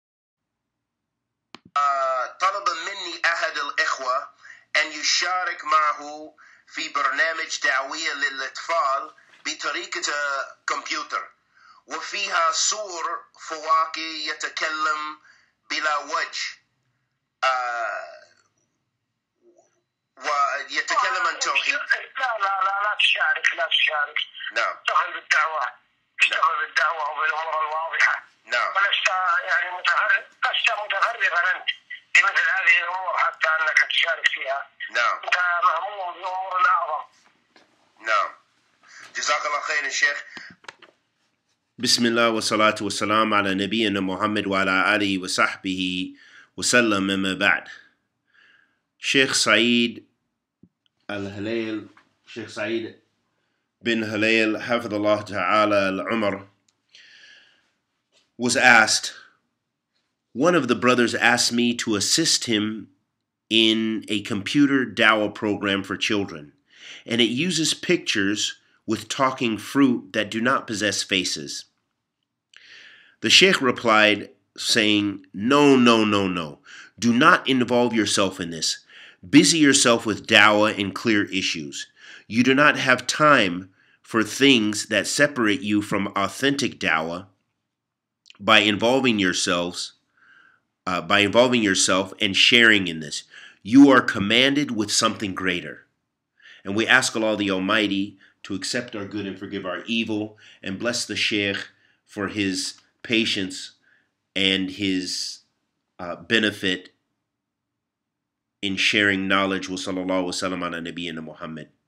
Should We Make Dawa Using Cartoons? Shaykh Sa'eed Halayl الشيخ سعيد بن هليل العمر
minhaj of dawa